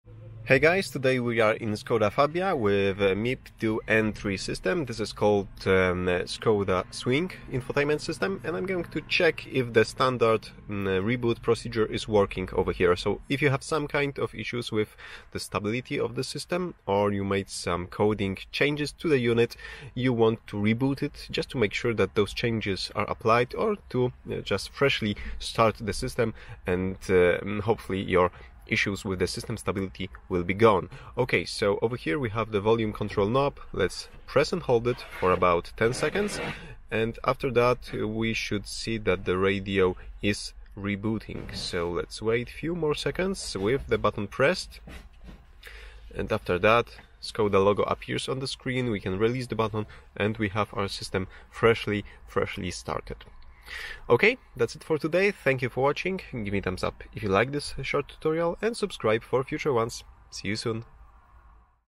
Skoda Swing MEN2 (MIB2 Entry) force system reboot
How to perform system reboot in Skoda Swing MEN2 (MIB2 Entry) Infotainment system after making coding/adaptation changes or to restart the system when experiencing some stability issues.

ℹ️ Locate the volume control knob, press and hold it for about 10 seconds. After that you should see Škoda logo on the display. This indicates system reboot. Release the button and wait for the MIB2 system to fully load all applications.

ℹ️ The same or similar system was installed in Škoda Fabia, Rapid, Karoq, Kodiaq.

0:00 Skoda Swing MIB2 Entry system
0:12 Restart reasons
0:31 System reboot procedure
0:49 System restarted